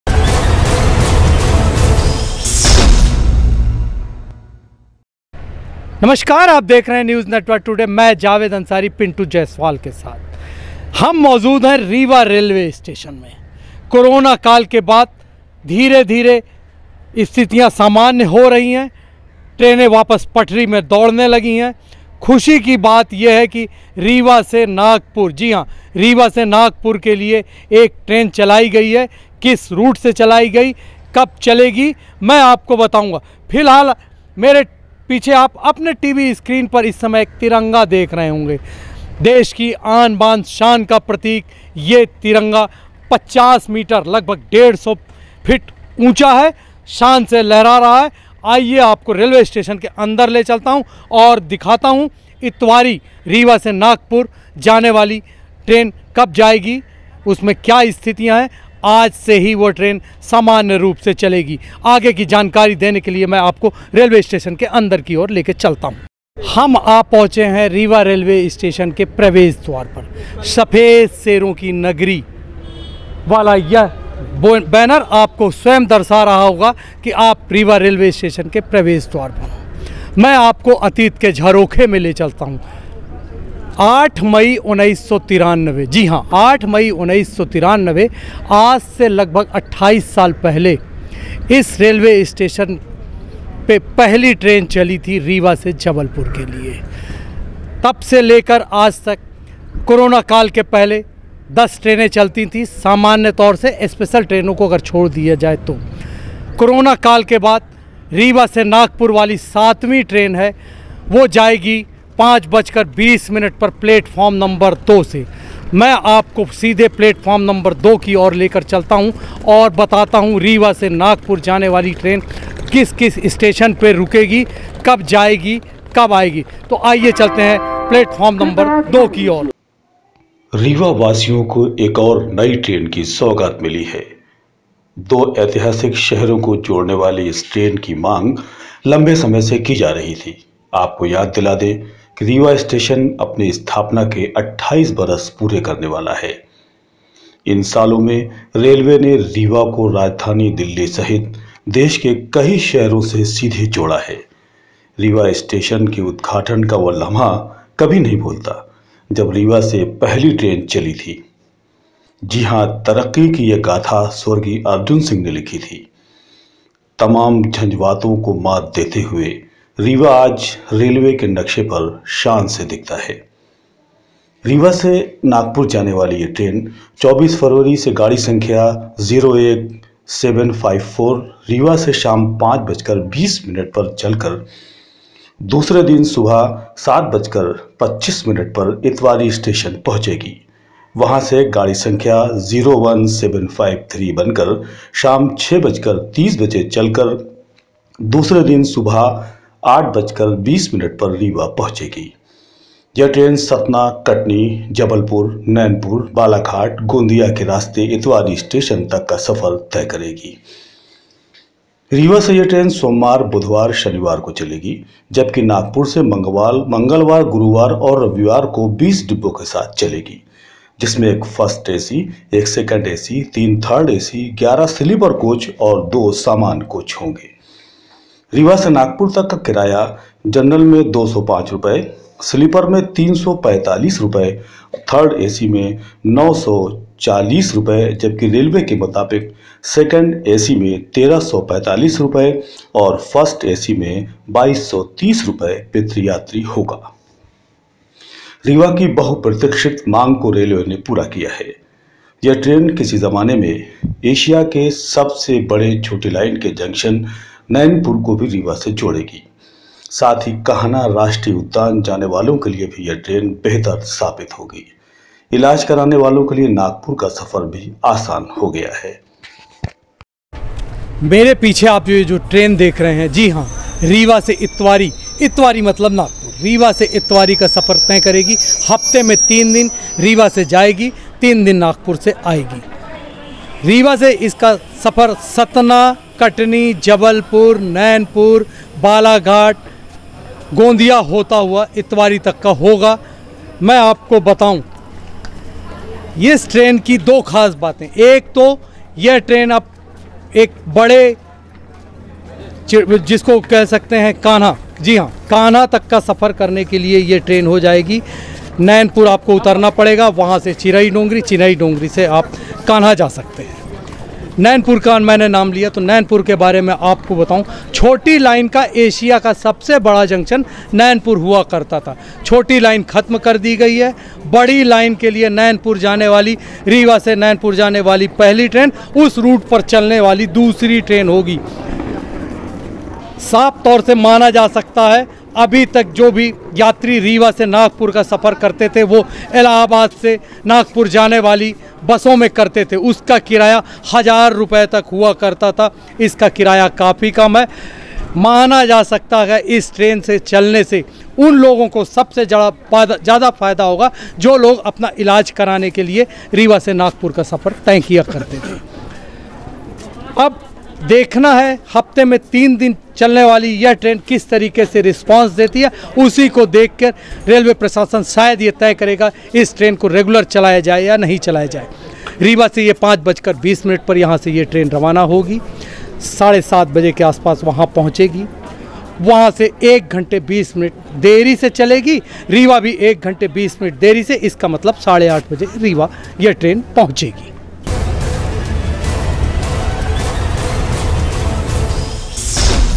रीवा नागपुर ट्रेन का नियमित संचालन ||NNT
रीवा नागपुर ट्रेन का नियमित संचालन

24 फरवरी 2016 रीवा से चलकर सतना मैहर कटनी जबलपुर नैनपुर बालाघाट गोंदिया के रास्ते नागपुर के इतवारी स्टेशन तक चलने वाली ट्रेन का नियमित संचालन प्रारंभ हो गया कुरौना काल के बाद रीवा स्टेशन से चलने वाली यह सातवीं ट्रेन है इसके पहले स्पेशल को छोड़कर रीवा से 10 ट्रेनों का संचालन होता था